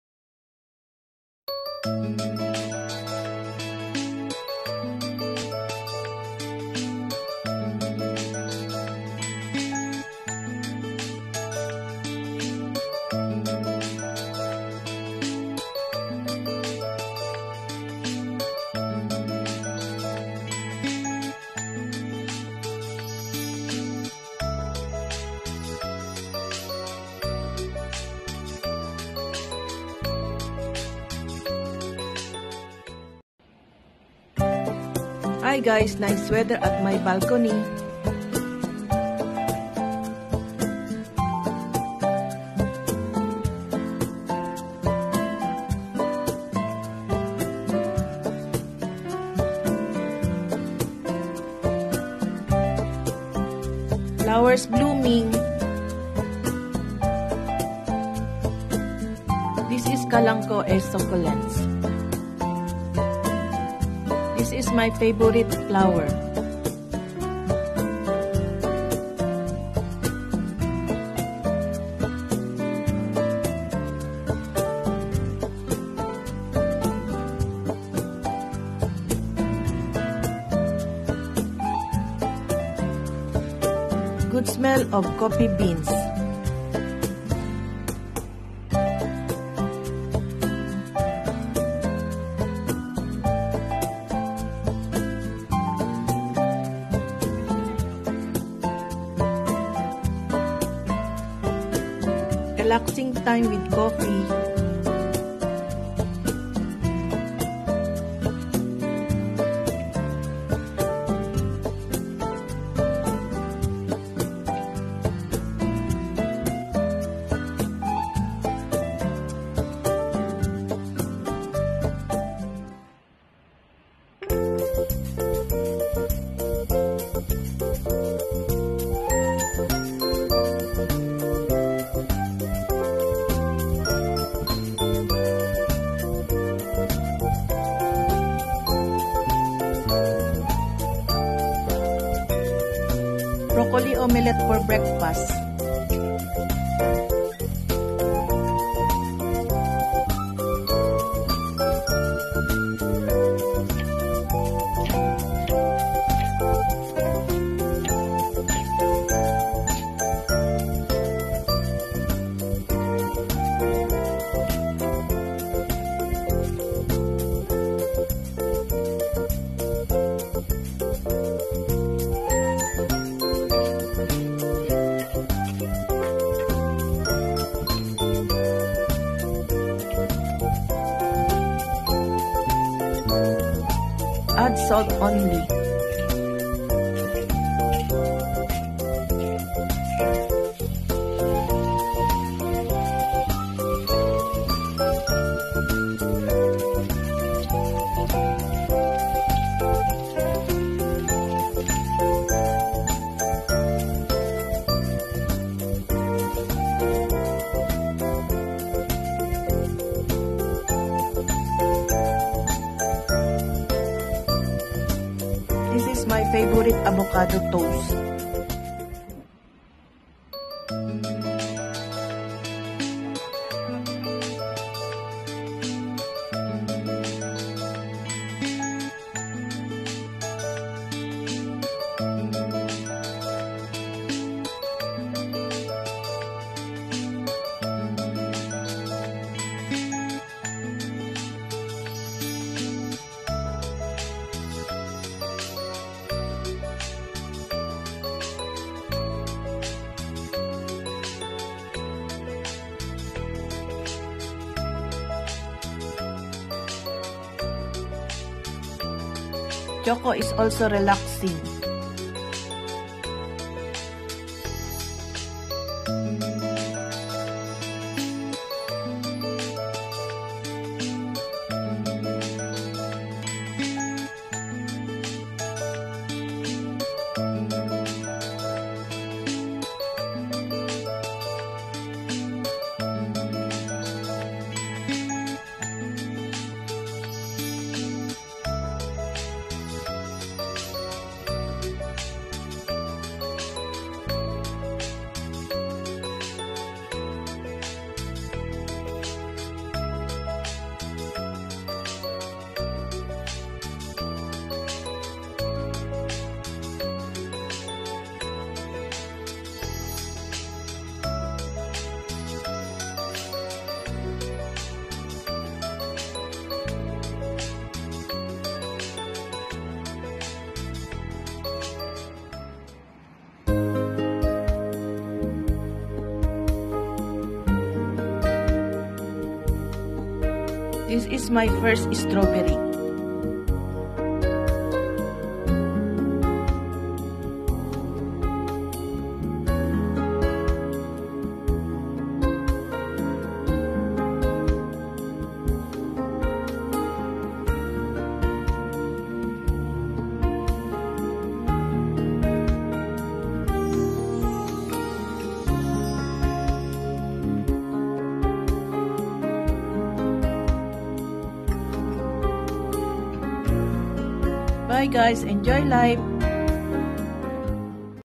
SPRING BALCONY GARDEN
spring @ my balcony my calanchoe and strawberry plants are blooming.